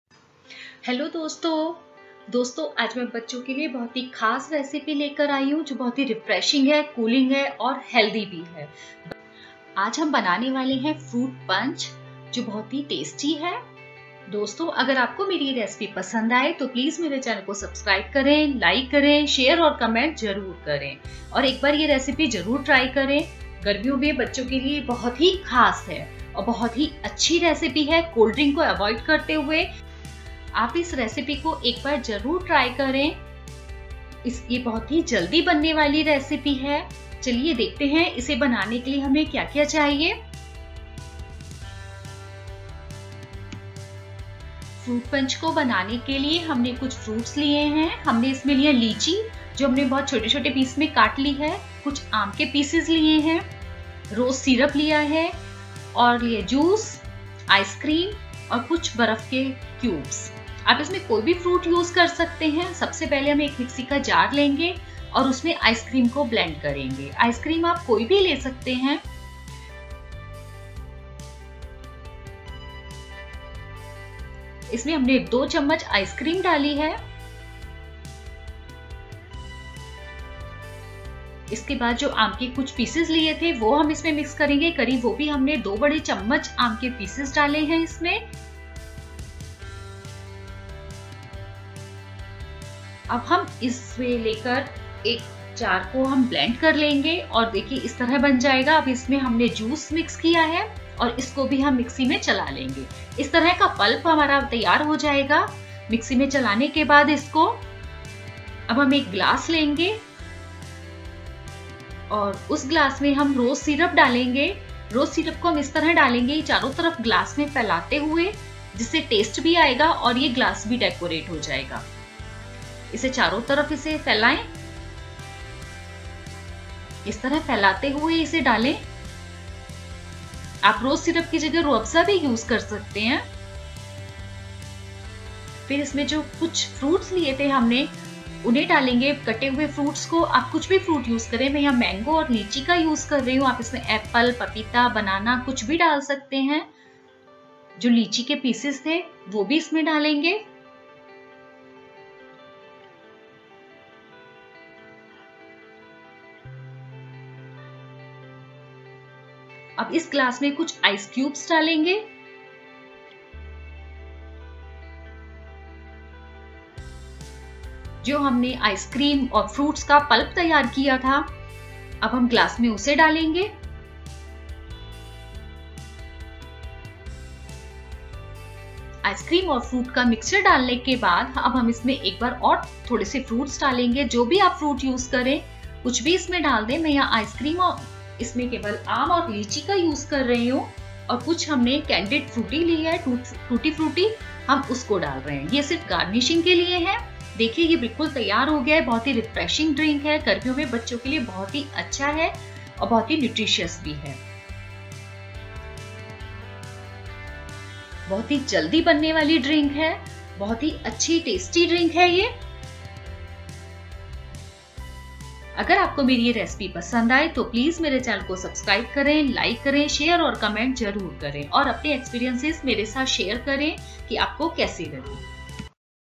Party Fruit Punch for Kids | Fruit Punch Recipe | Mix Fruits Punch Recipe | Mixed Fruits juice
This is the Classic fruits punch and very easy to make at home.. This recipe is specially for kid and for the parties.